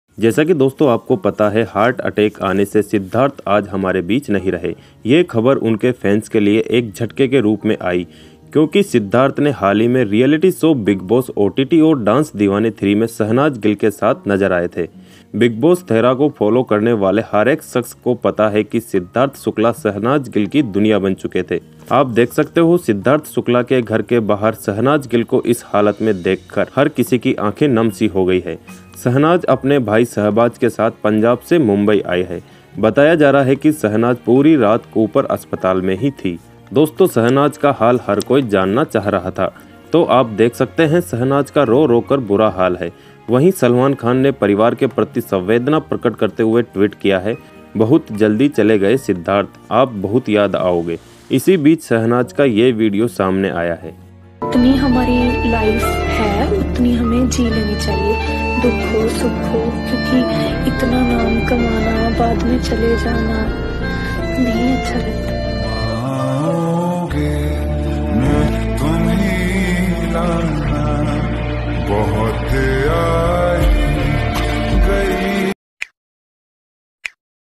Siddharth Shukla के जाने के बाद shehnaaz gill का हुआ बुरा हाल | salman khan भी रोने लगे
credit - Siddharth Shukla, shehnaz gill